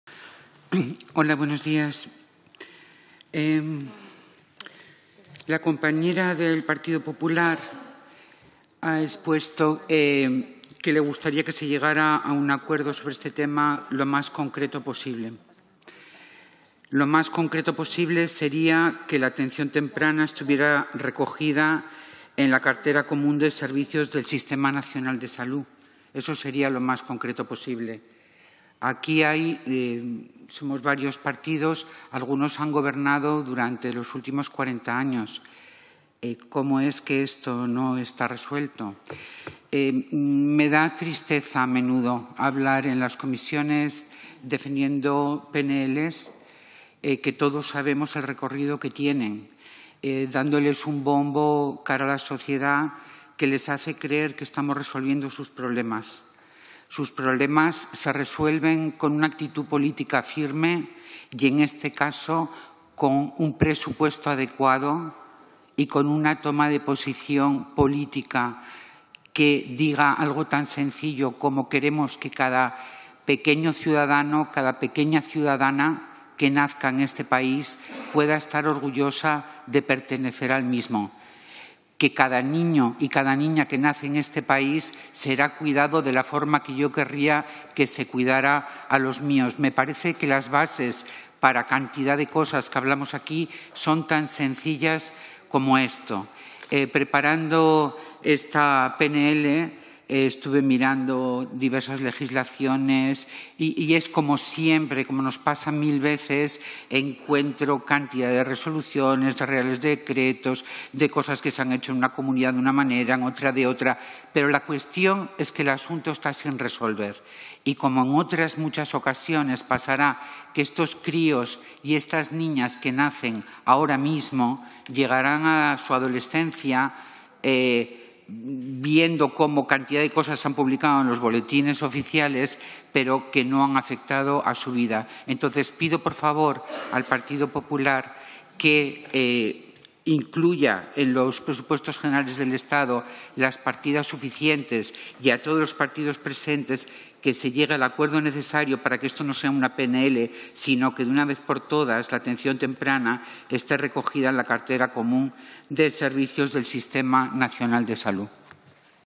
Mae de la Concha en la Comisión de Derechos de la Infancia y Adolescenecia 25 Octubre
Sobre la necesaria armonización de la Atención Infantil Temprana.